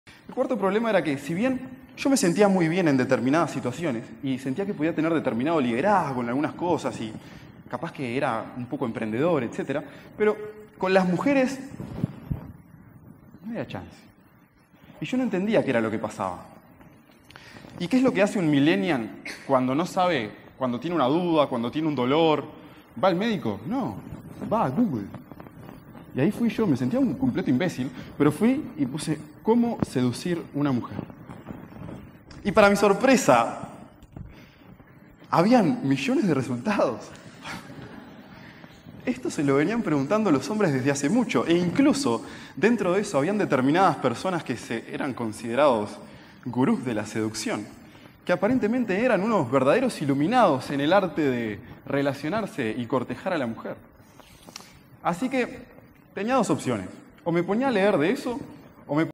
Matías Love - Charla TEDX
Si les soy sincero durante casi un año soñé con dar una Charla TEDx, un día me llega una llamada diciendo que yo era uno de los posibles candidatos para una charla, llegó por sorpresa, no me sentía preparado, pero era imposible dejar pasar esa oportunidad.

Hoy me siento super contento de que la charla se esté volviendo ciertamente popular, de las más vistas de mi país.